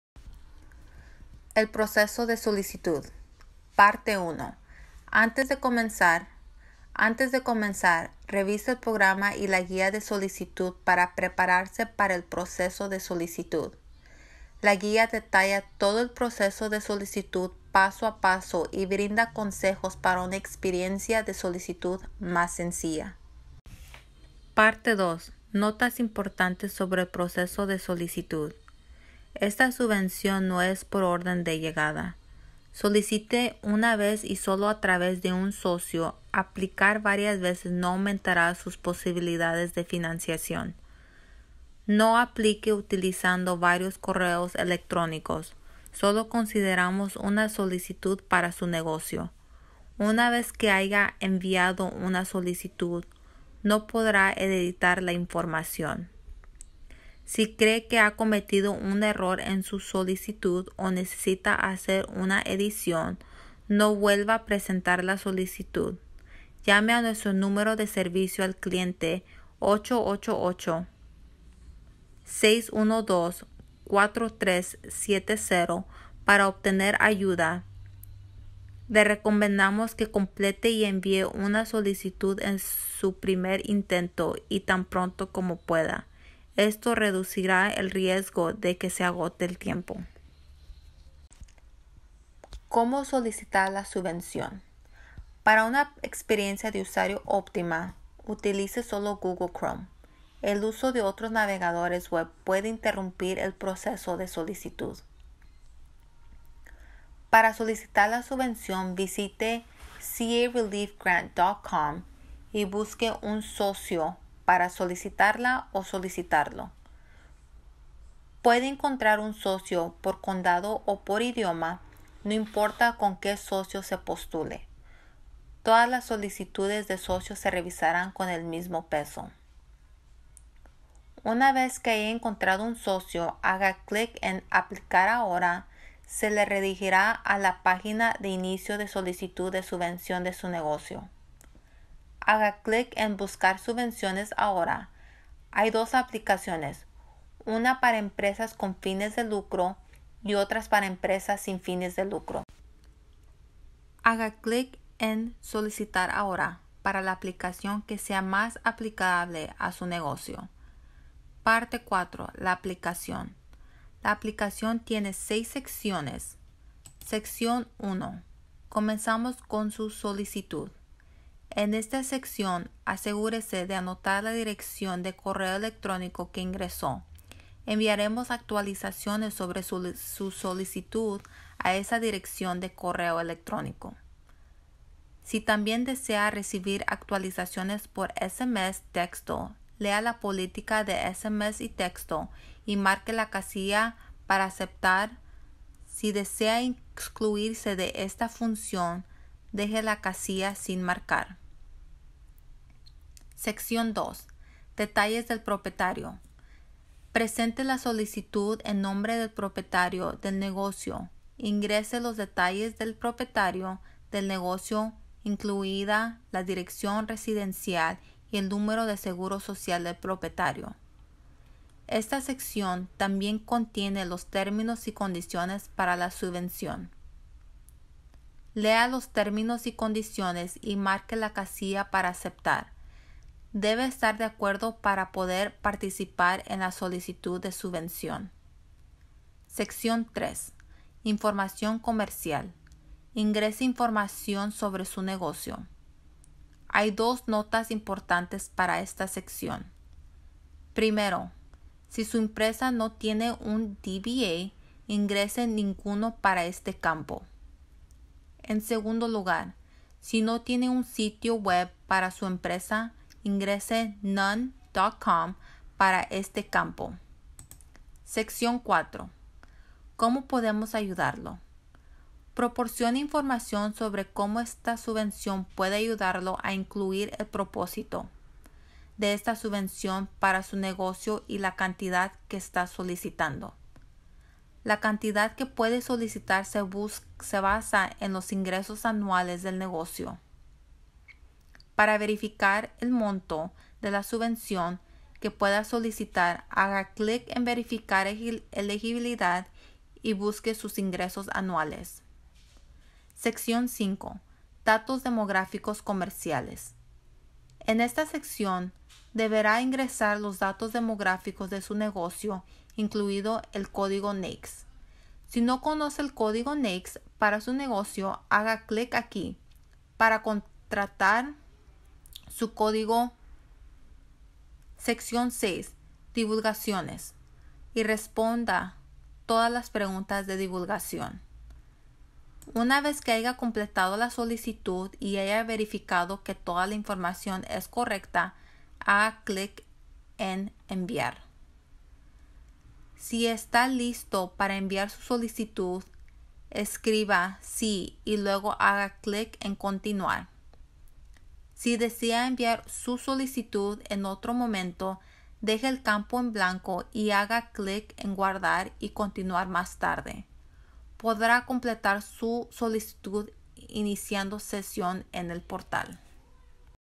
Video en Español, CA Relief Grant Program: Como applicar
El gobernador Newsom y la legislatura estatal han anunciado la asignación de $ 500 millones para la pequeña empresa de California.
La fecha límite para aplicar es el 13 de enero de 2021.